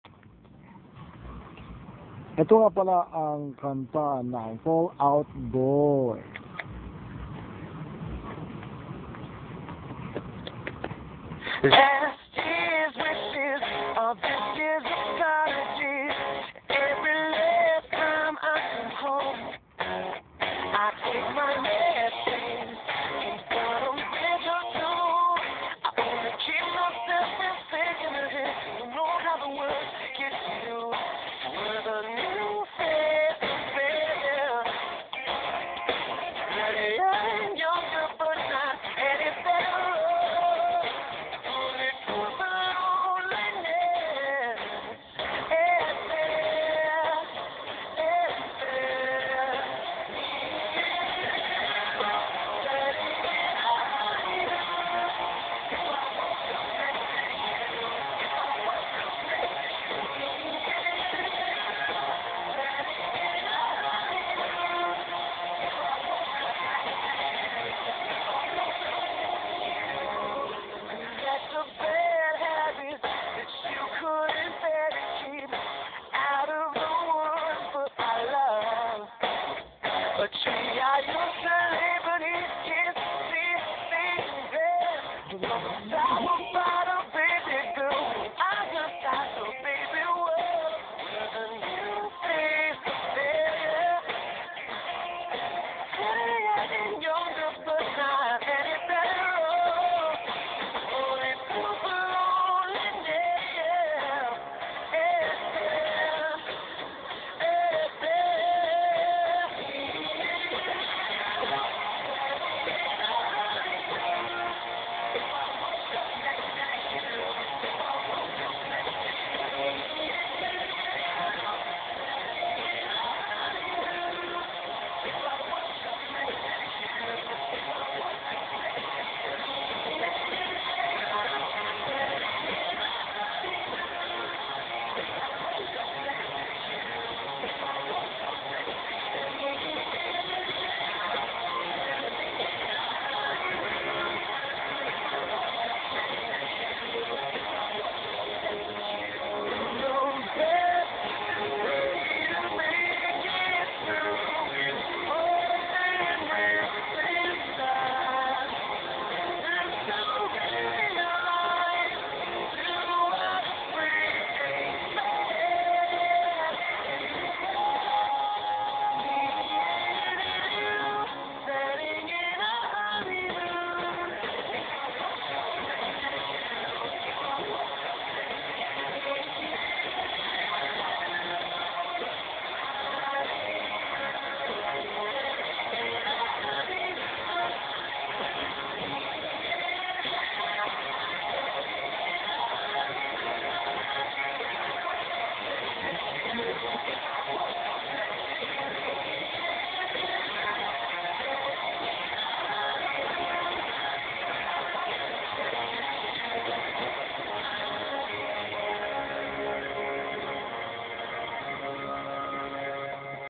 ang gawa naming cheer dance
ang pinag hirapang pag proctisan ng madaming araw, mga taga st vincent angmga ito